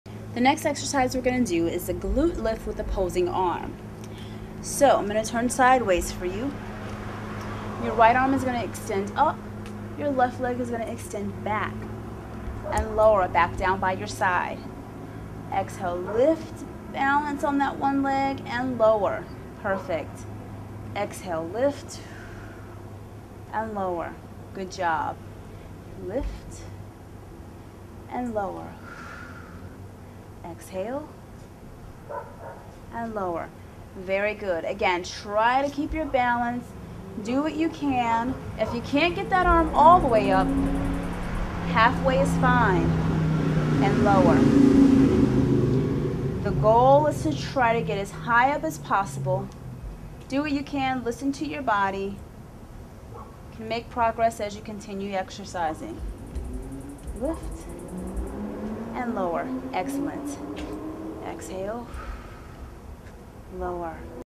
Glute Lift with Opposing Arm Exercise for Senior Health & Fitness
Glute Lift with Opposing Arm Exercise for Senior Health & Fitness. Part of the series: Senior Fitness Exercises. Seniors, learn how to gain balance using the glute lift with opposing arm exercise for seniors in this free exercise video lesson for senior health and fitness.